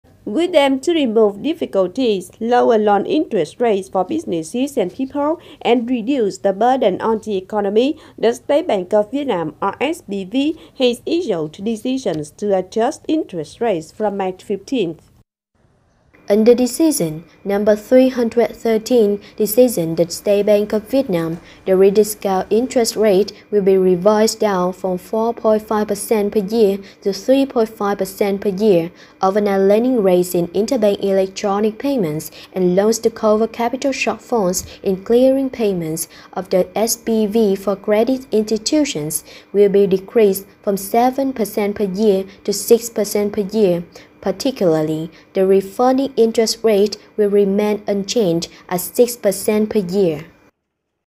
State Bank of Việt Nam decreased regulatory interest rates from March 15th | Cần Thơ News
With the aim to remove difficulties, lower loan interest rates for businesses and people, and reduce the burden on the economy, the State Bank of VN (SBV) has issued decisions to adjust interest rates from March 15th.